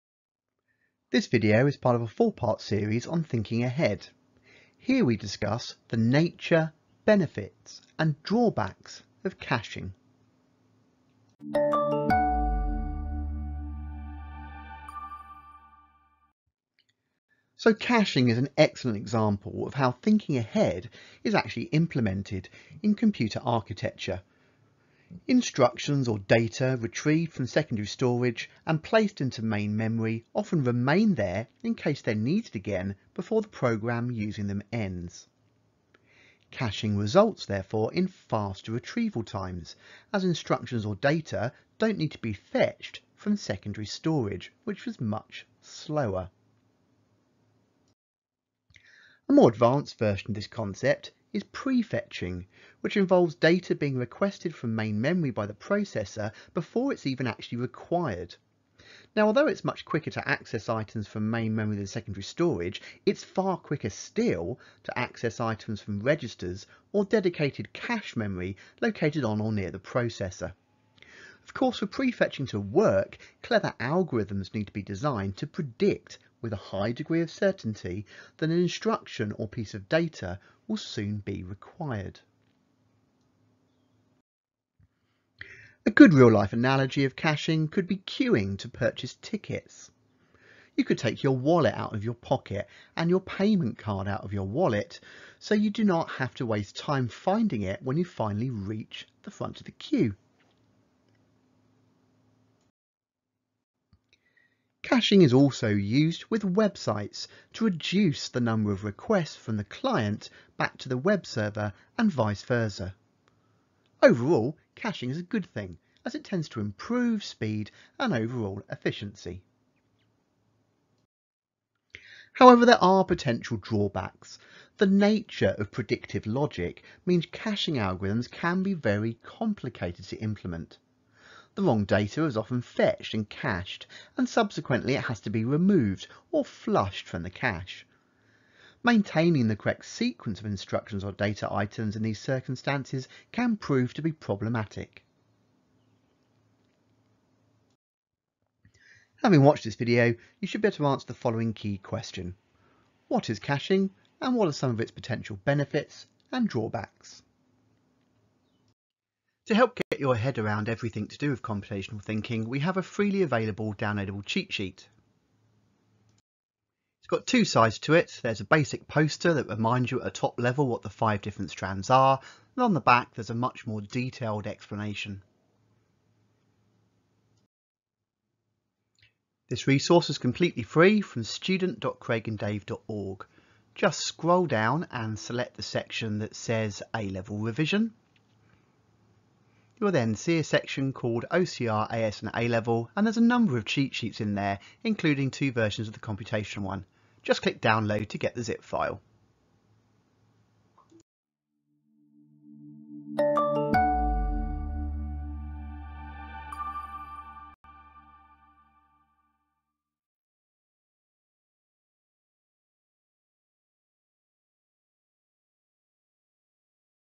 122. OCR A Level (H446) SLR19 - 2.1 Caching
A Level 2.1.2c

00:00 Intro
00:16 Caching
01:24 Analogy
01:43 Caching Positives and Potential Drawbacks
02:28 Key Question
02:39 Computation Thinking Cheat Sheet
03:21 Outro